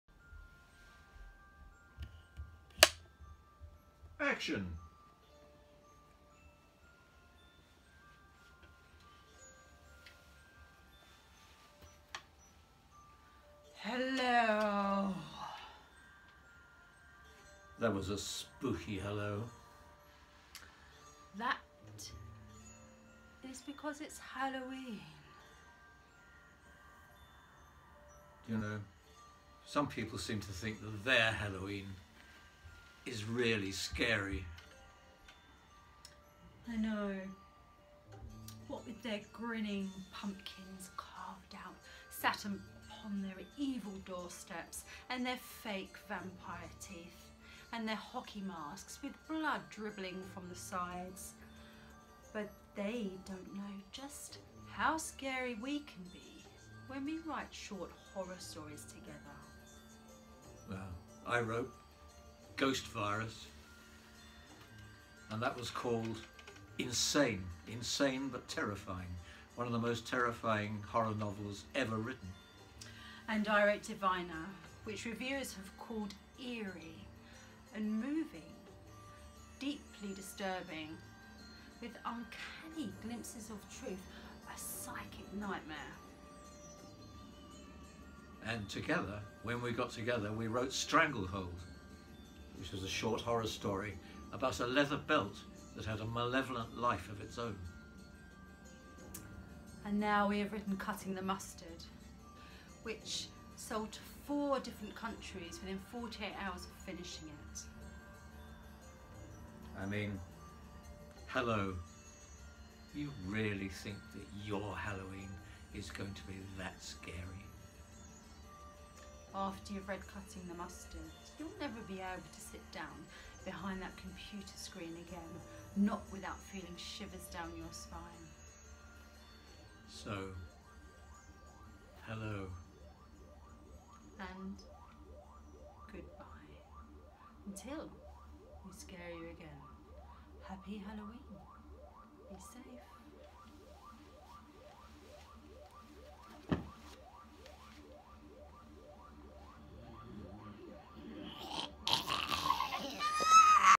Graham Masterton and Dawn G Harris Halloween Promo
Graham: 'Dawn and I have made a short promotional video for Halloween. Our new horror story CUTTING THE MUSTARD will be published in a new anthology from Jeani Rector who runs the very popular monthly online blog The Horror Zine.'